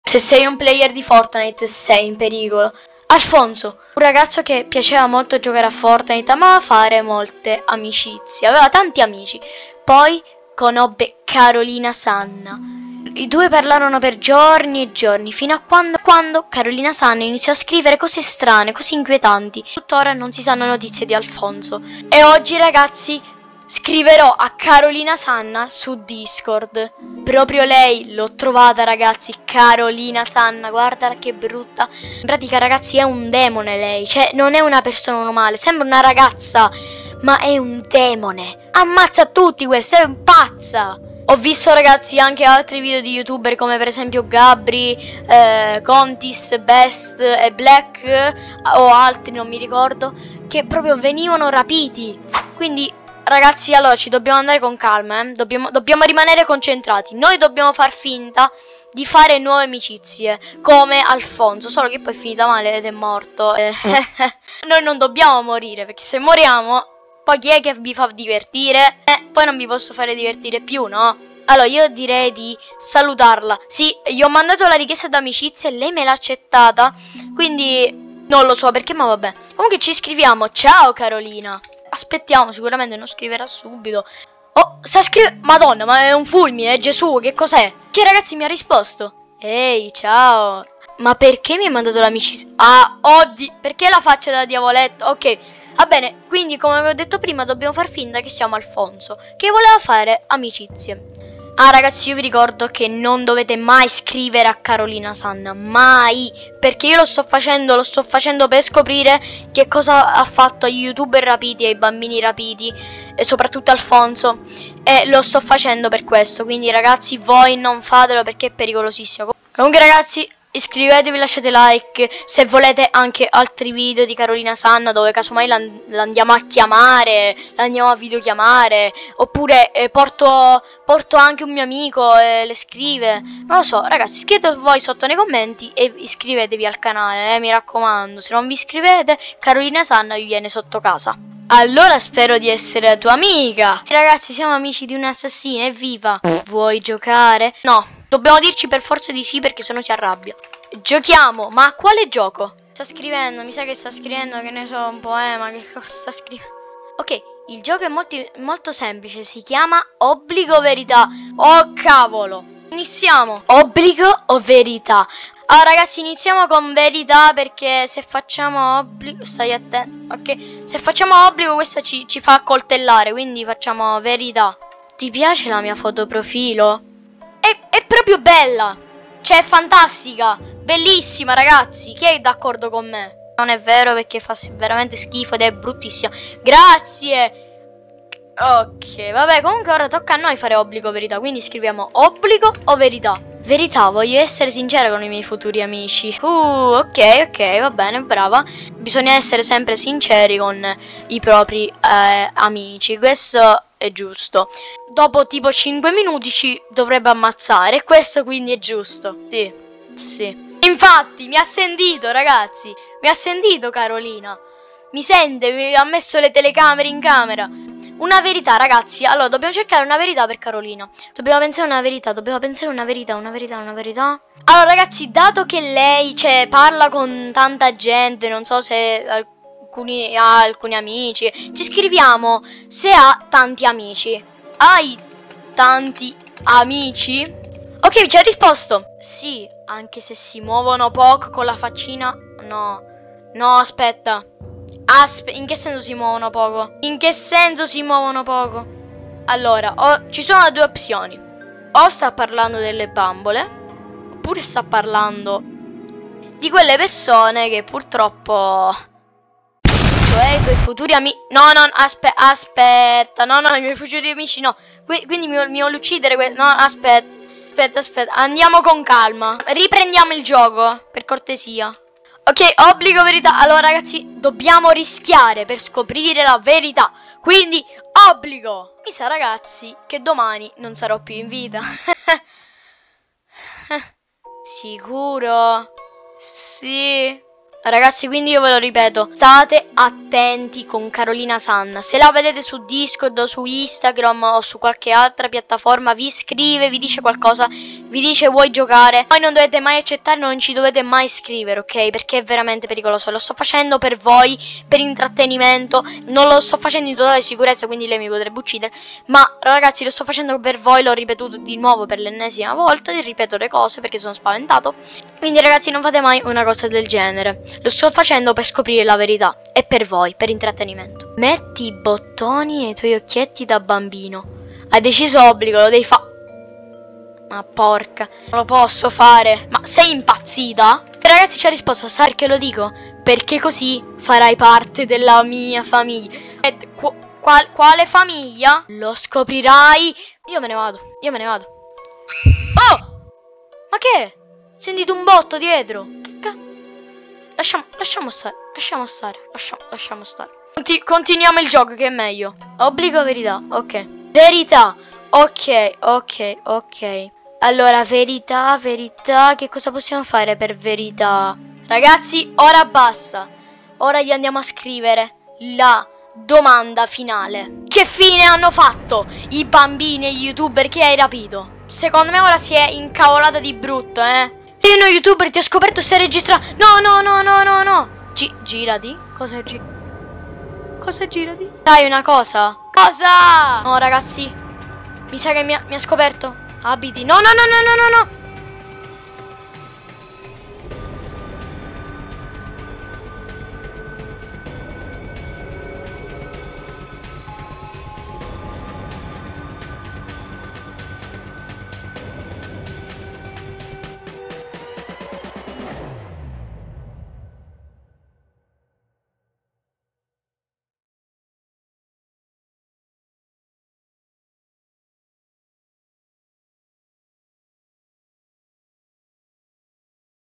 CAROLINA SANNA è ENTRATA IN CASA MIA!!! (SONO IN PERICOLO!) | ITA
Aiuto! Carolina sanna mi vuole UCCID. In questo video ho rischiato davvero tanto. Se vedi Carolina Sanna su DISCORD, INSTAGRAM o su qualche altra piattaforma, NON SCRIVETECCI MAI E NON PARLATECI MAI, perchè Potrebbe essere MOLTO PERICOLOSO.IL VIDEO, L' HO SOLO PER INTRATTENIMENTO, VOI NON REPLICATE ASSOLUTAMENTE LE COSE CHE HO FATTO. FATE ATTENZIONE!!!